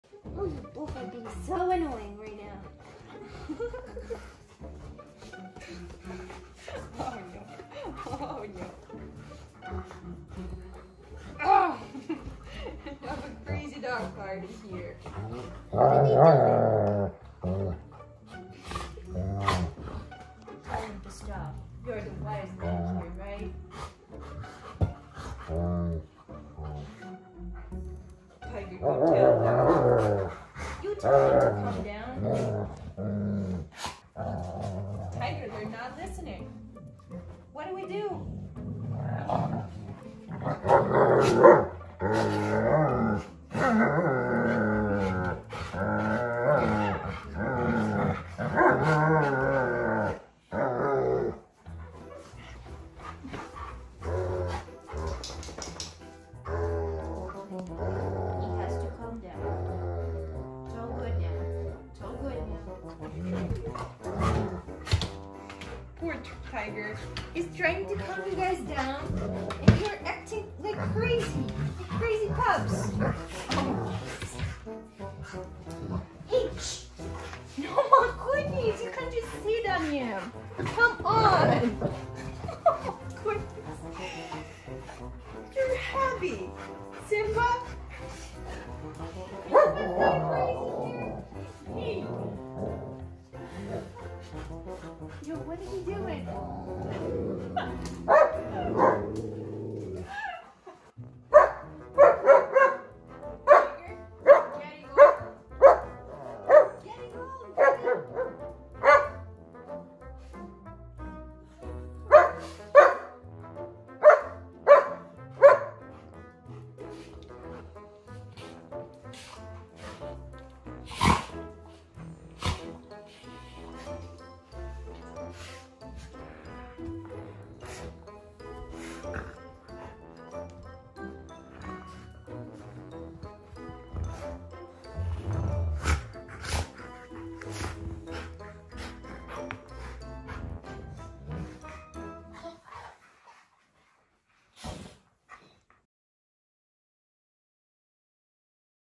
German Shepherd Steps In As PEACEMAKER Every Time My Dogs Fight
Tiger, our German Shepherd is the peacemaker in our house. Every time my Golden Retrievers start a fight, he steps in and calms them down.